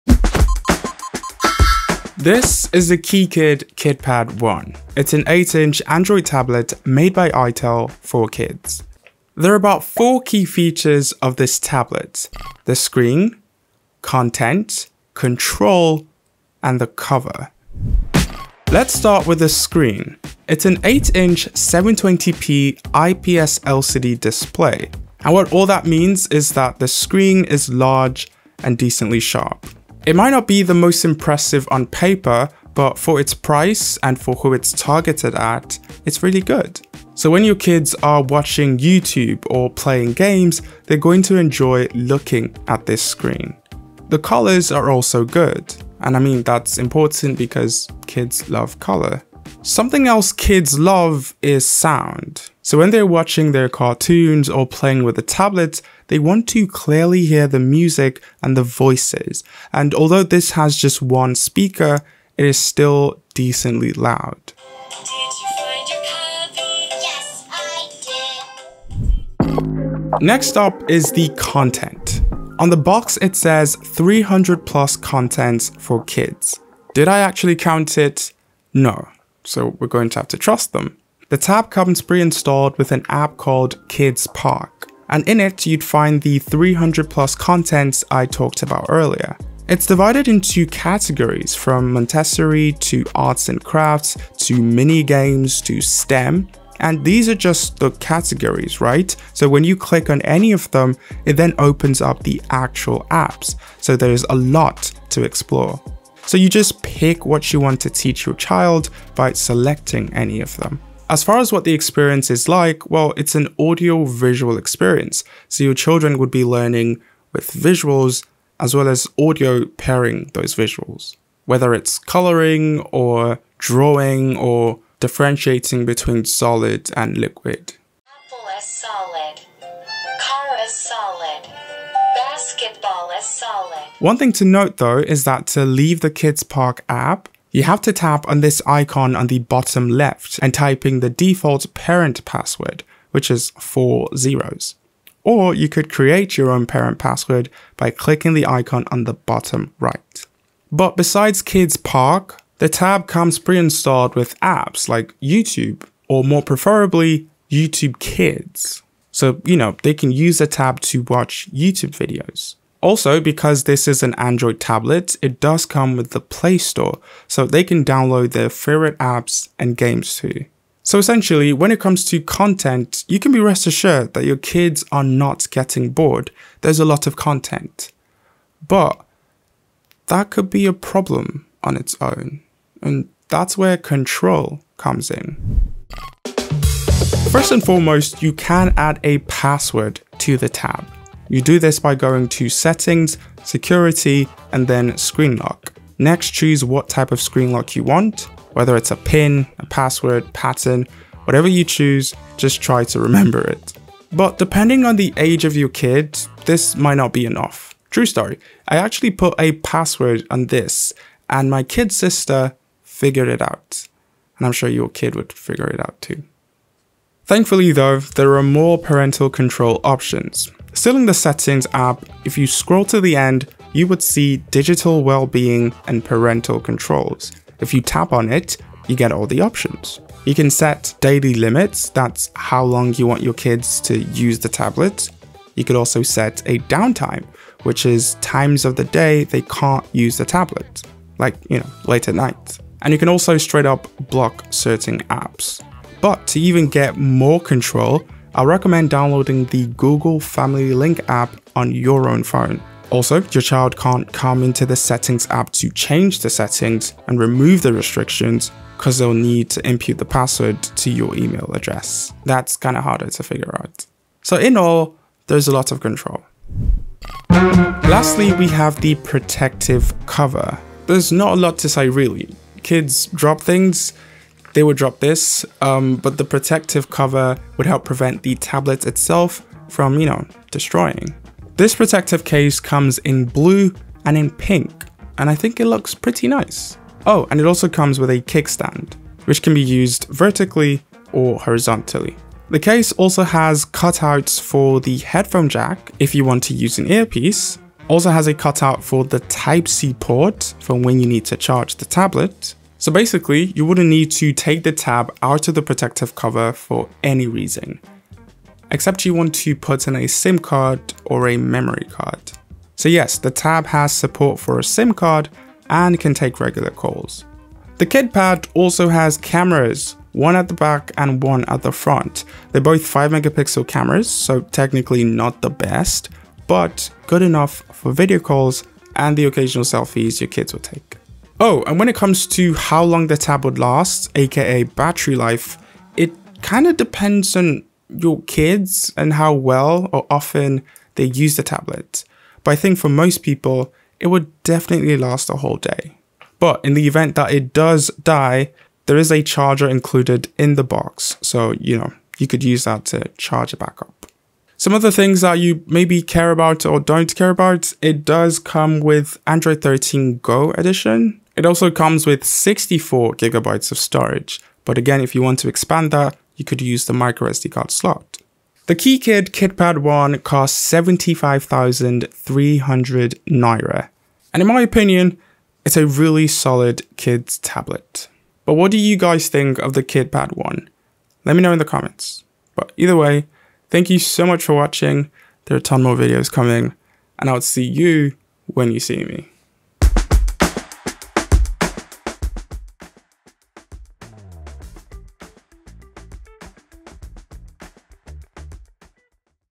itel KeeKid Kid Pad 1 Review - Budget Kid Tablet !
Itel KeeKid Kid Pad 1 Review.
This is itel’s first entry into the kid tablet space and I can say for about $95, it comes with a lot of the necessary features you would want in a tablet for your kid. This is the first tablet I’ve reviewed on the channel, it was a little different and also fun. Hope you found the video useful and you can definitely reach out if you have any more questions.